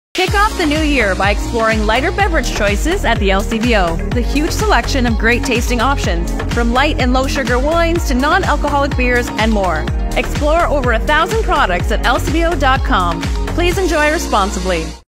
Our very own Angela Aielo (aka @superwinegirl1982 ) shares some drier options available at the LCBO
The perfect way to kick off the new year with drier (low-alcohol, non-alcohol,, low-sugar) choices available at the LCBO (@lcbovideo ).